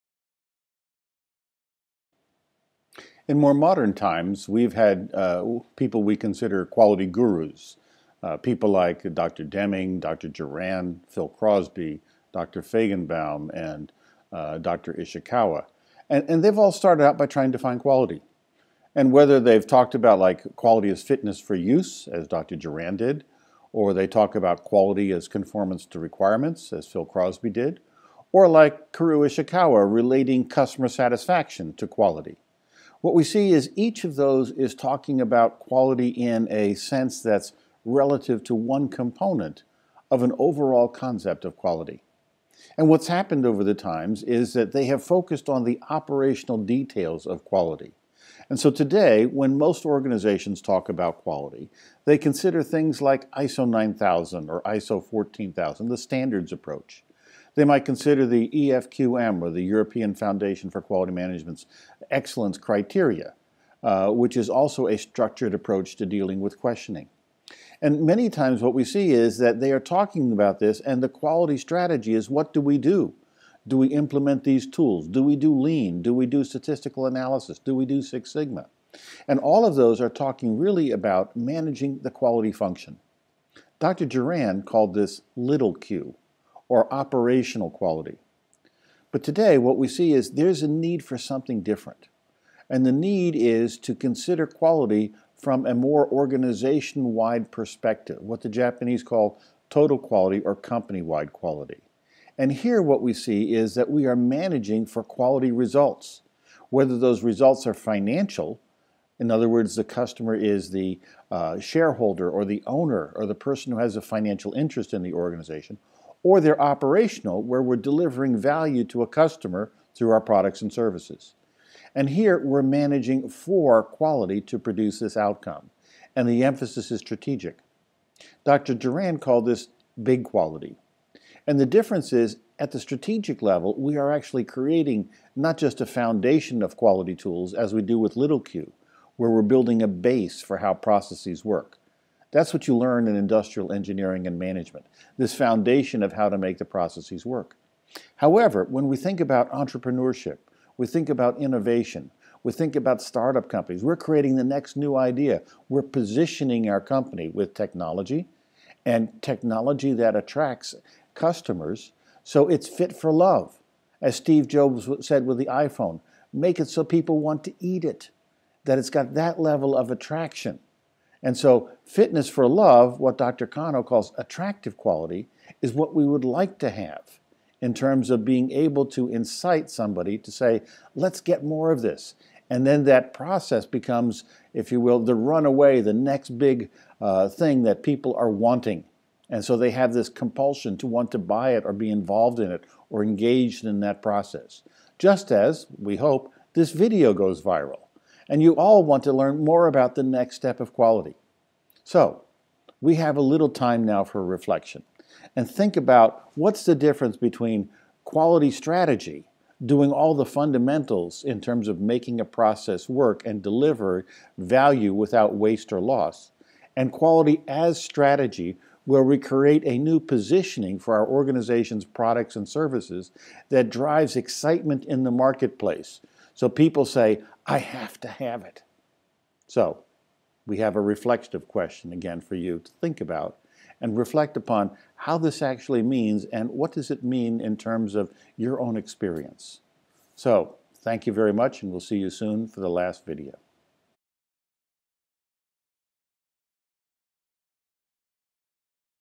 Quality as Strategy | What Do We Mean by Quality | Part 3 | Gregory H. Watson
Is there a difference in having quality strategy or Quality as strategy?
Today there is a need to consider quality from a company wide perspective.
Japanese call this “Big Q” as Total Quality or Company Wide Quality.

Gregory H. Watson is the first non-Japanese individual to receive the W. Edwards Deming medal from the Union of Japanese Scientists and Engineers (2009). He is also a recipient of the highest award of the American Society for Quality - the Distinguished Service Medal (2011) and the highest award of the European Organization for Quality - the Georges Borel Medal (2013).

Greg is considered a pioneer in the Six Sigma movement for participating in the early development while Director of Corporate Quality at Compaq. He earned his MBB from Mikel Harry in 1998 while acting as Deployment Champion for introduction of Six Sigma at Nokia Mobile Phones. while ASQ President, he was the initiator of the ASQ Six Sigma Black Belt certification. He is also the author of over 10 publications, and has had long-term assignments with companies such as HP, Xerox, ExxonMobil, and Toshiba.

To go directly to the materials of the course (85+ videos and 9 ppt with training sessions) visit